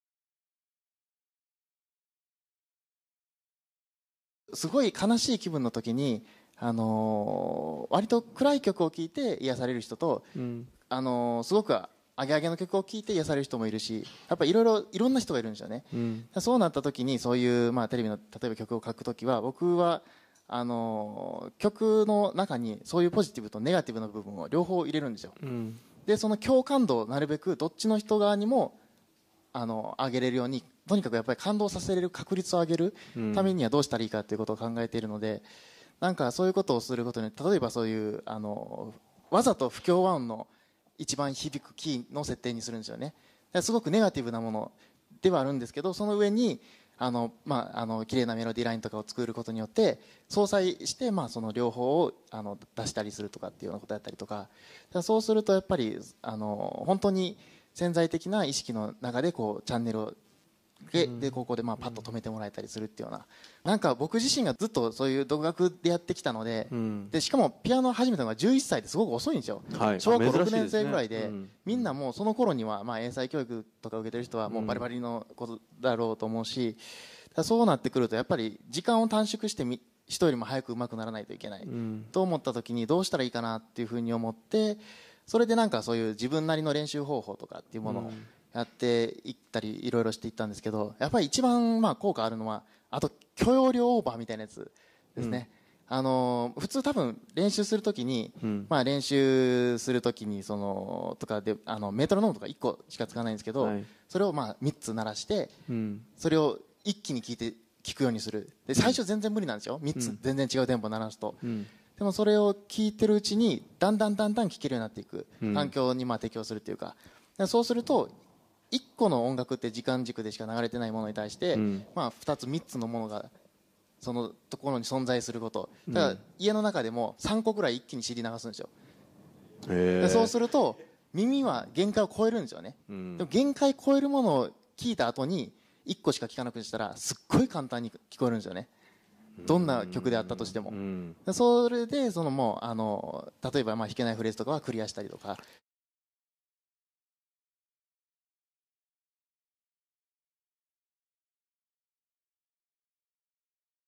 意識を広げるために自ら許容量オーバーさせよう
ピアニスト・松永貴志氏
あすか会議2015
第6部 分科会「クリエイティビティと経営」　ダイジェスト版　Part2/2

クリエイティビティはもはや経営の一資源ではなく、競争優位の源泉となりつつあり、画一的なマス・マーケティングから、経営者が自らの美学やクリエイティビティを市場に問う時代が到来している。ジャズピアニストとして世界で活躍する松永貴志氏が語る、「クリエイティブ」になるために大切なこととは？あすか会議2015のダイジェスト版（視聴時間2分34秒） 。